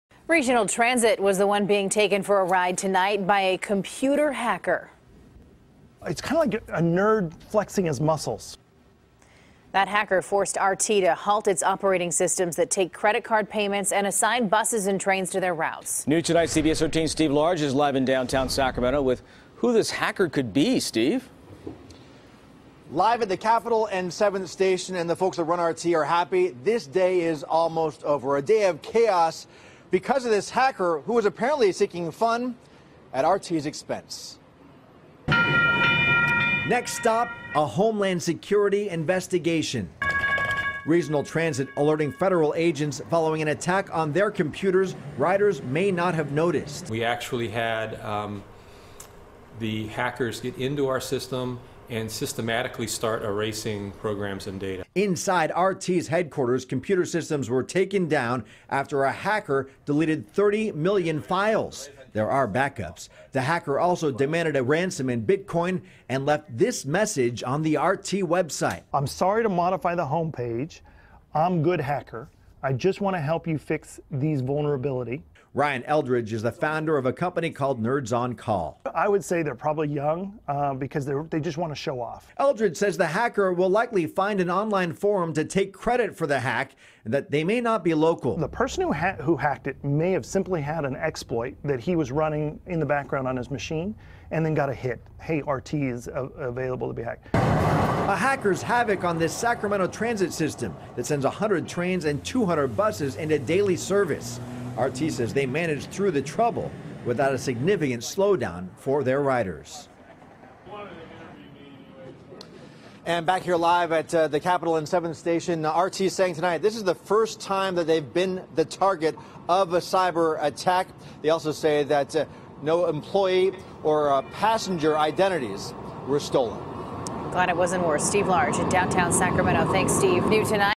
Sacramento Regional Transit Systems Hit By Hacker
That hacker forced RT to halt its operating systems that take credit card payments, and assigns buses and trains to their routes.